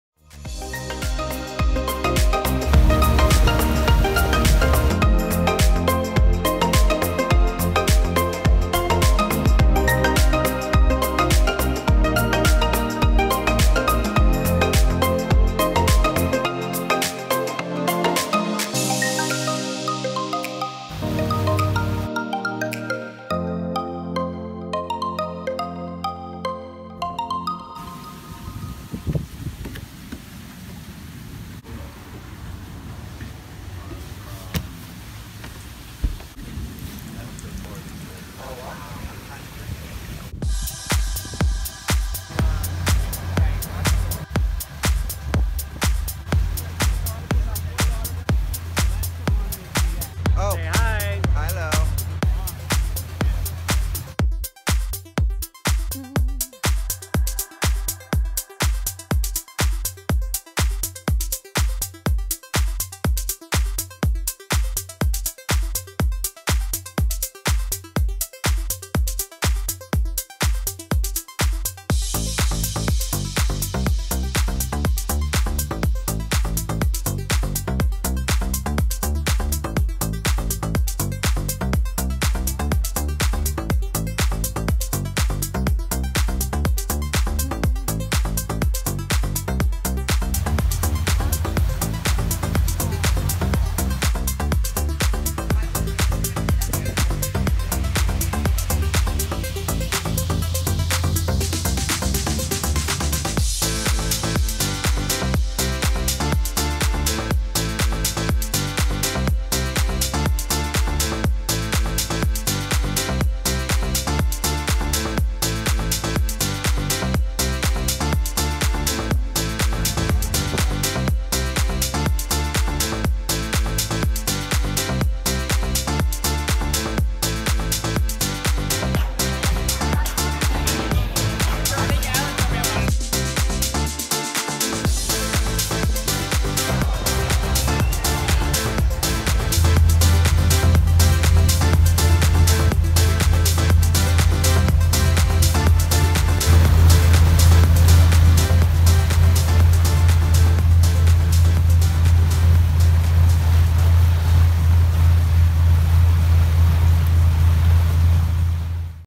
FORT LAUDERDALE BOAT RIDE 02/04/18
Take a ride with me as we tour the Fort Lauderdale intercoastal.